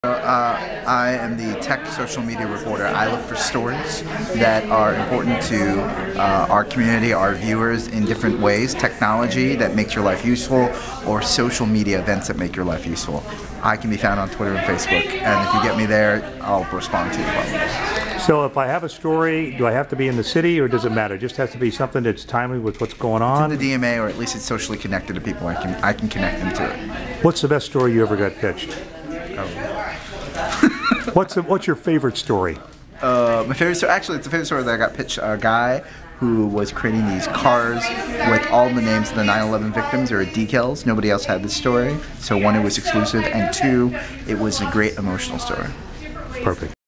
NBCLA's Mekahlo Medina loves exclusives, socially engaging pitches in Social Media
Medina was a panelist at the first PRSUMMITLA, Culver City, CA talking about pitching media in real time.